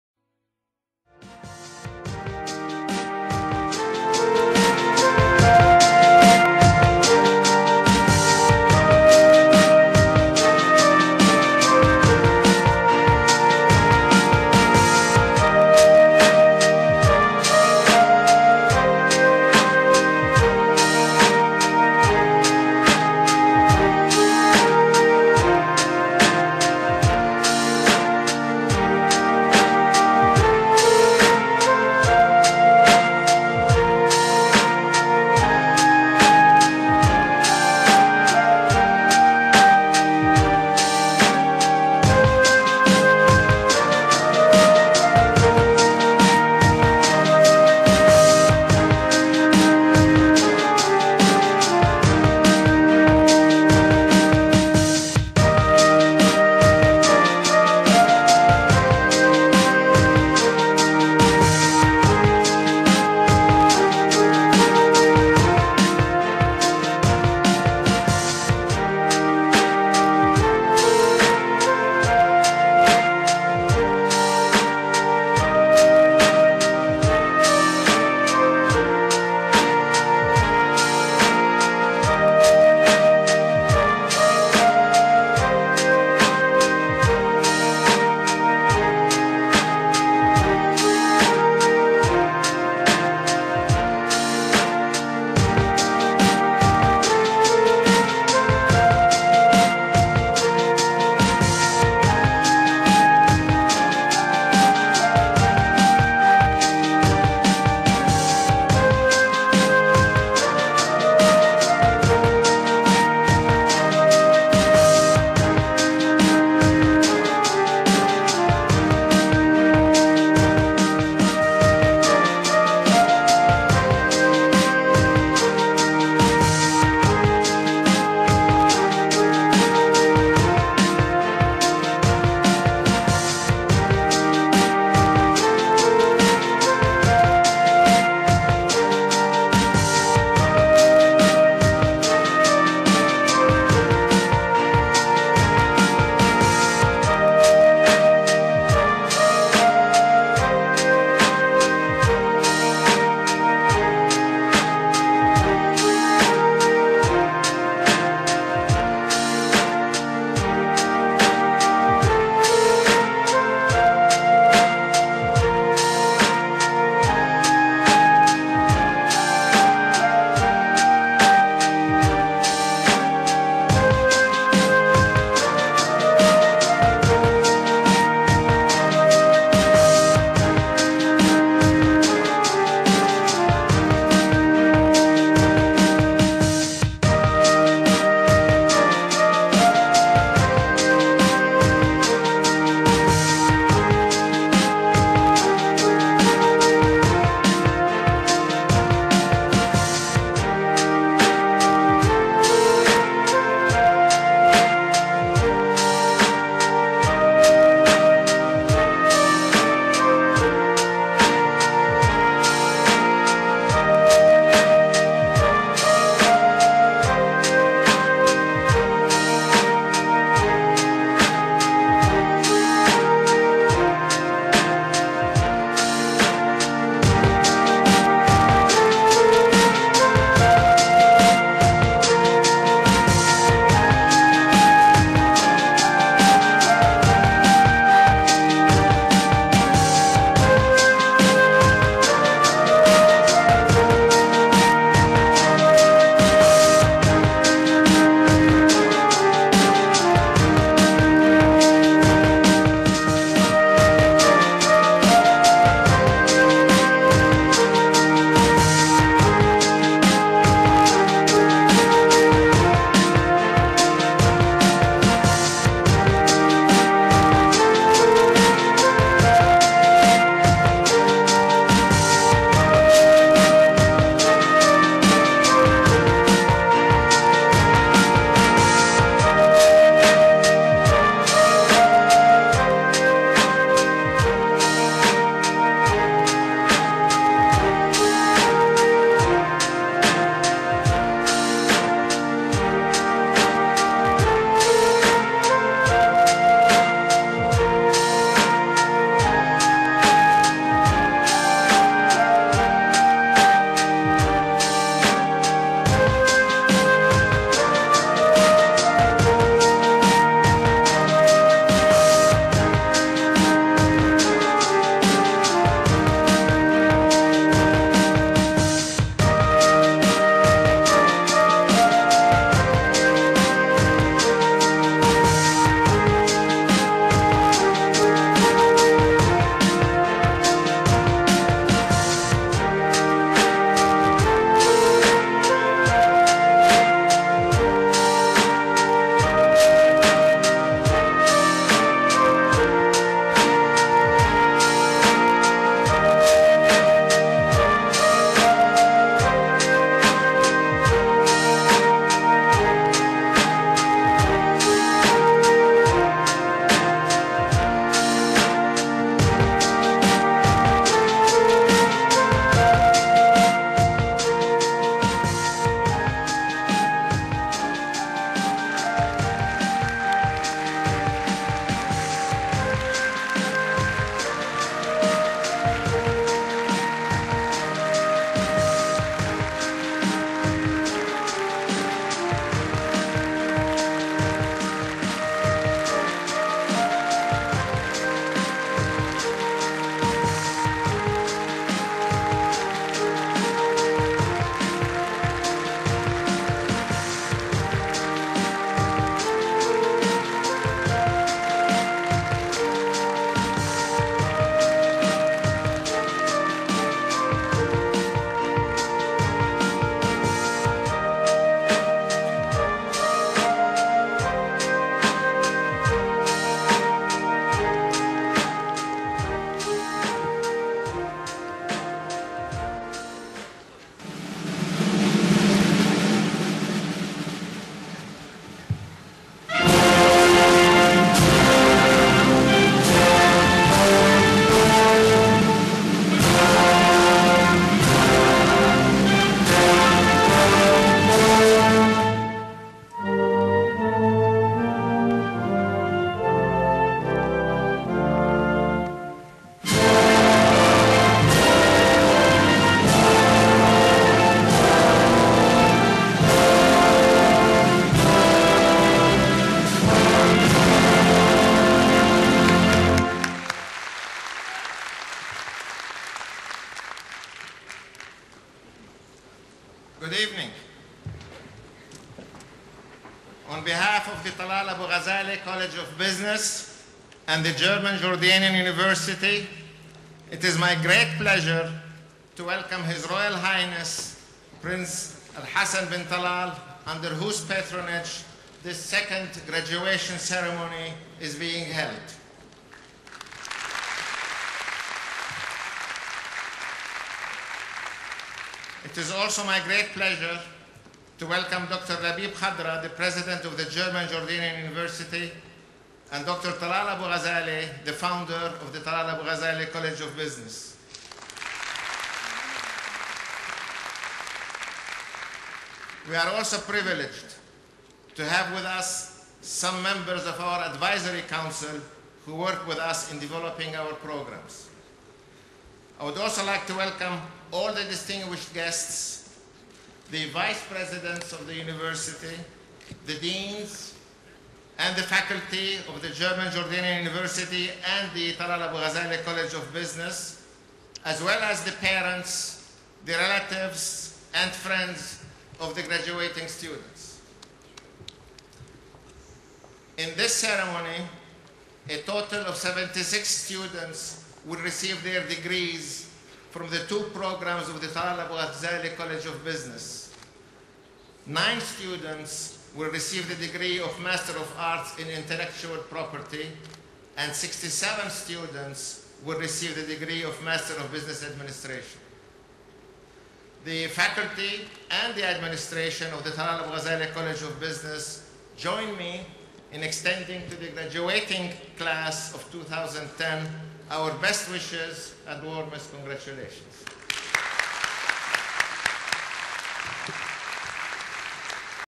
The Graduation Ceremony of the second Masters class of TAG-College 1/9
The Graduation Ceremony of the second Masters class of the Talal Abu-Ghazaleh College of Business /German Jordanian University under the patronage and presence of HRH Prince El Hassan bin Talal and Dr. Talal Abu-Ghazaleh, Head of the TAG-College Advisory Board - July 22, 2010.